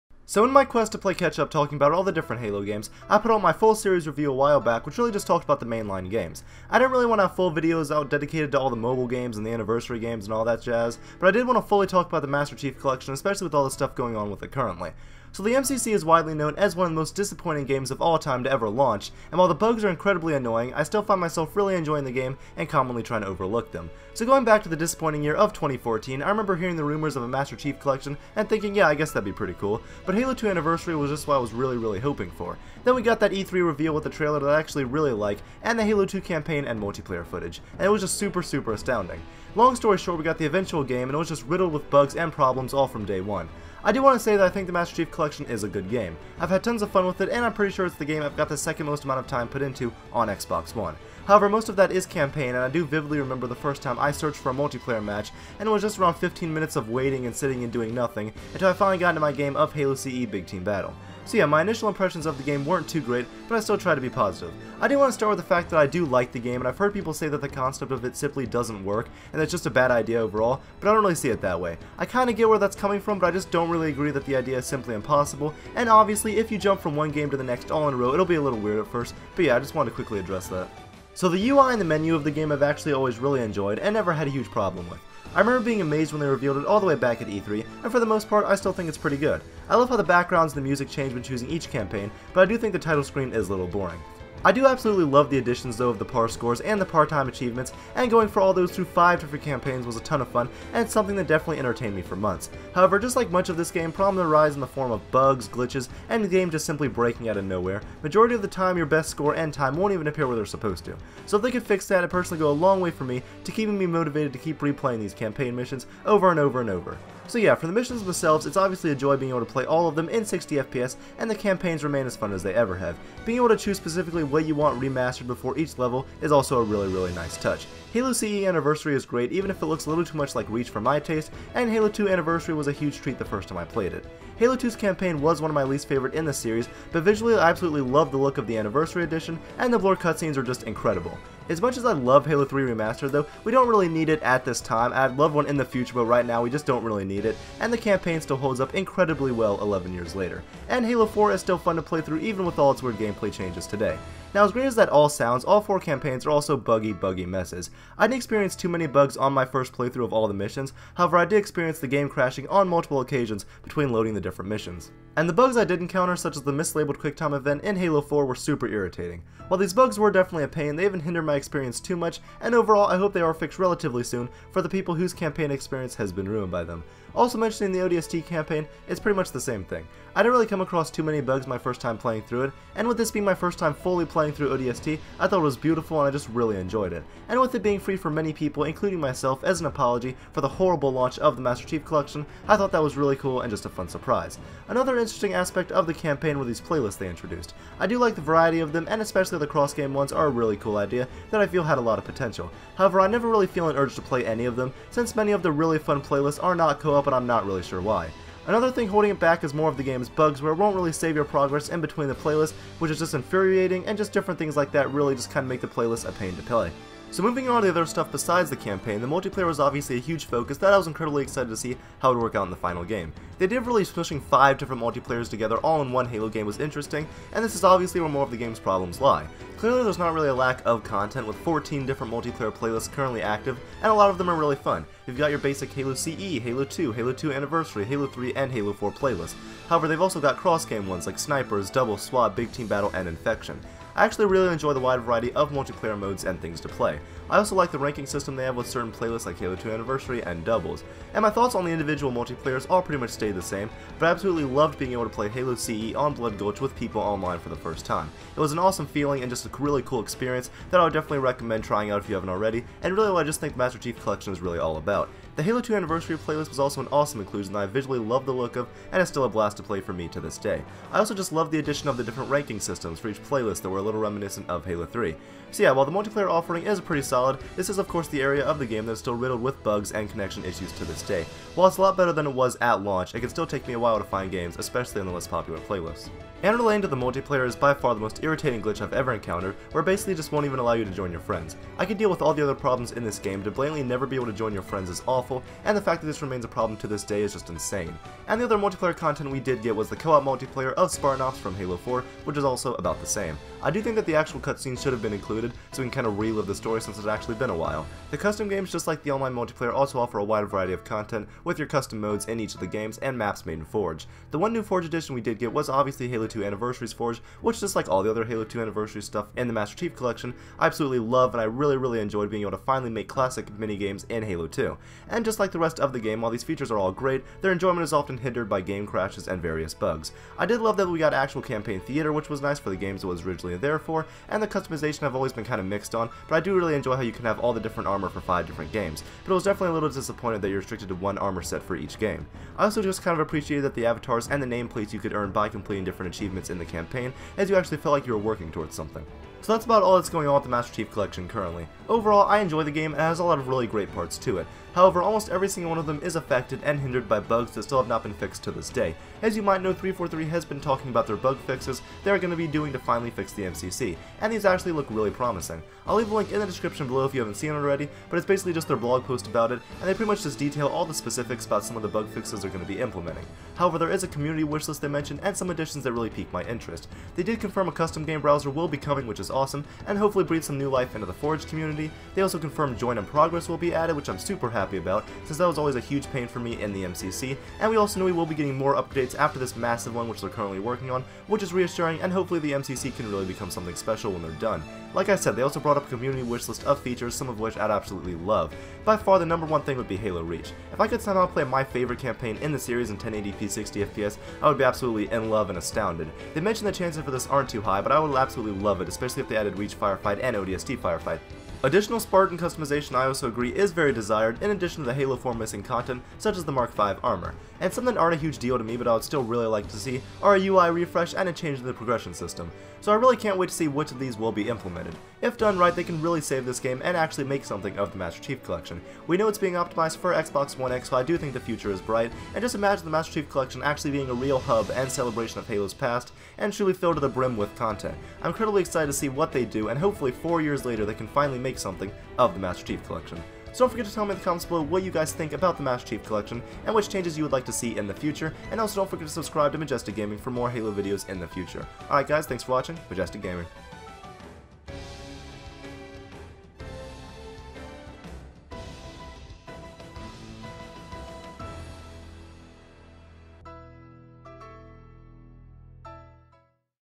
Is the Master Chief Collection a GOOD Game?
Today, I bring you guys my thoughts on Halo: The Master Chief Collection in addition to what I want from the future updates!